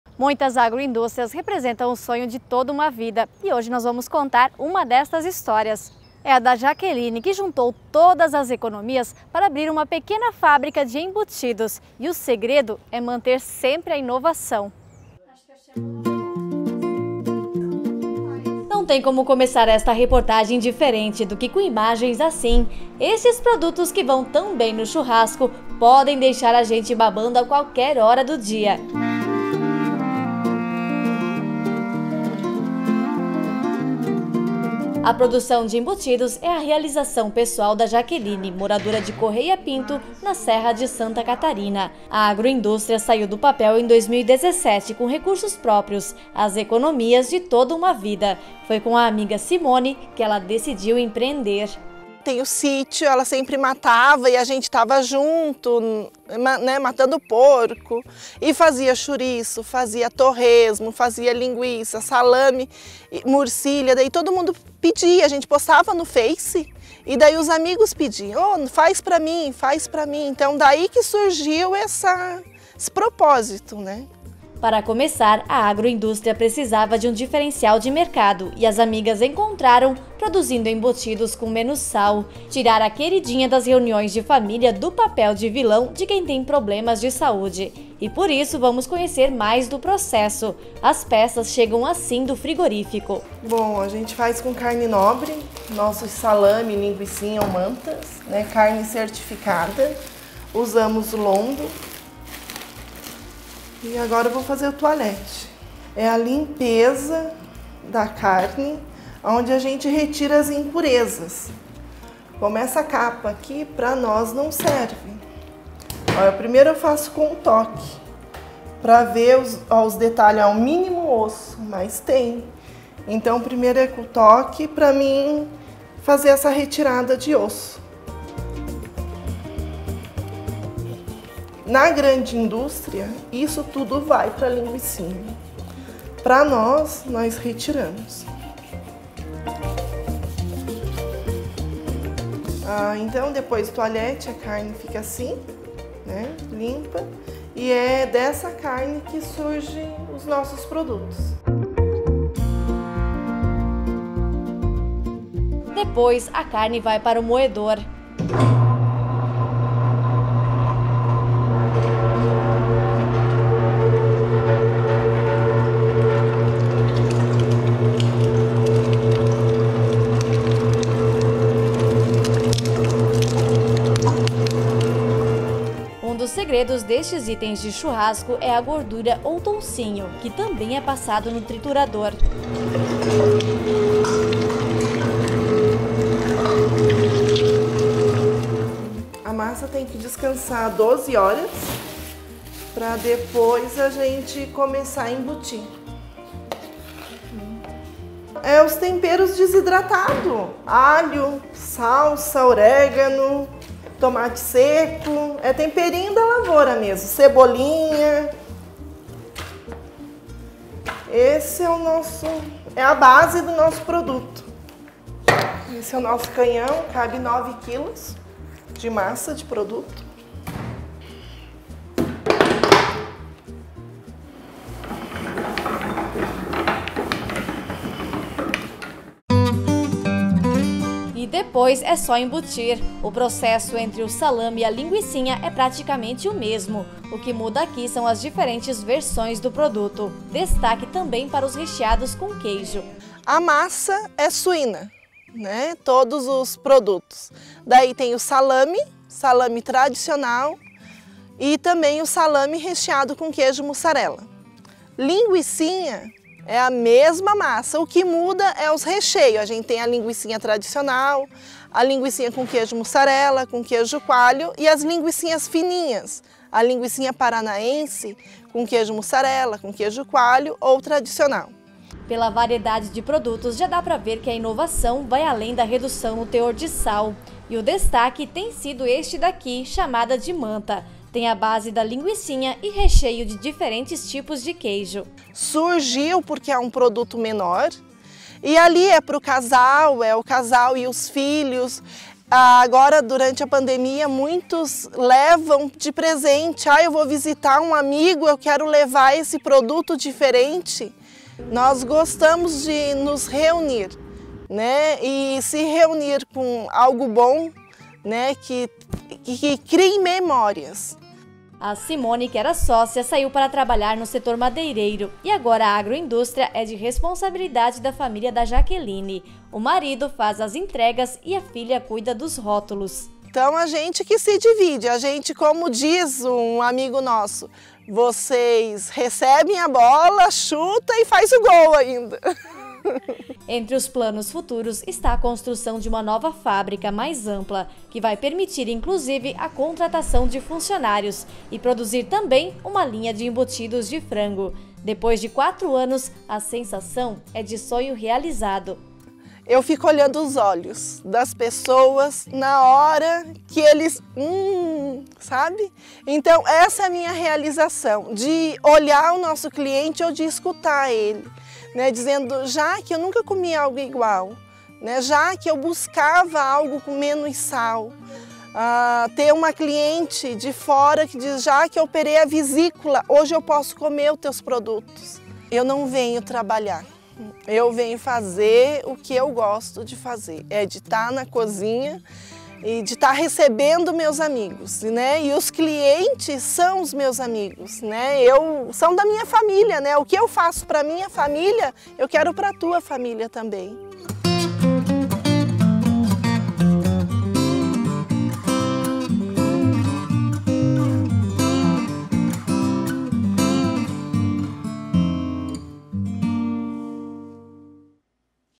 COM AS ECONOMIAS, ELA CONSTRUIU UMA AGROINDÚSTRIA DE EMBUTIDOS!
AGROINDÚSTRIA DE EMBUTIDOS | Conheça a história da Jaqueline, que com as economias da família construi uma fábrica para produzir embutidos artesanalmente. Produtos com menos sal, são nicho de mercado!

Mais informações - Lingmone Defumados - Correia Pinto (SC)